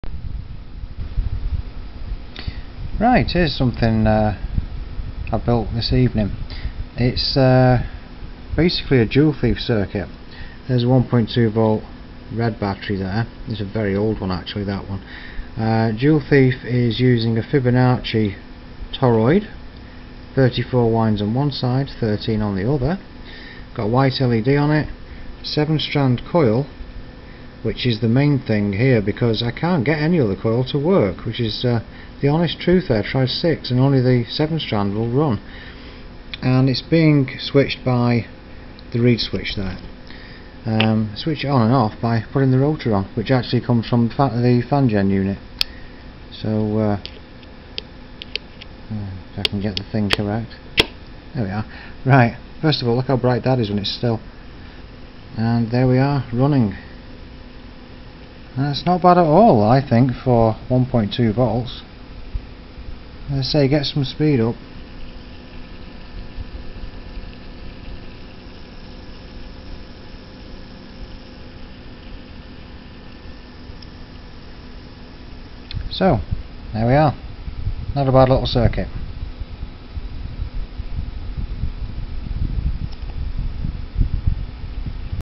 Joule Thief motor - 7 strand coil
See description:

A standard Joule Thief circuit, using a 2SD879 transistor, 1K resistor, white LED.
It features a trial of the Fibonacci toroid that i've come up with. 34 turns on one side, 13 on the other. The join of the two is the positive battery connection.
The copper gauges are intended to create an equal amount of copper in both windings,
The circuit also has a 7 strand coil on it, as recently experimented with in other videos. Only this coil will work on the circuit...i;ve tried about half a dozen others, from self wound to relays, none work for some reason except the 7 strander.

The reed switch controls the switching and is connected to the positive of the battery, such that, the whole circuit switches on and off rather than just the coil. It means better energy usage and also allows switching the circuit off by removing the rotor.
Rotor is from the FanGen unit and is heavy, with 6 North outward facing magnets.

Power used at 1.2V is 25mA

Experiments will follow to see what may be able to be collected from the collapsing rotor powering coil.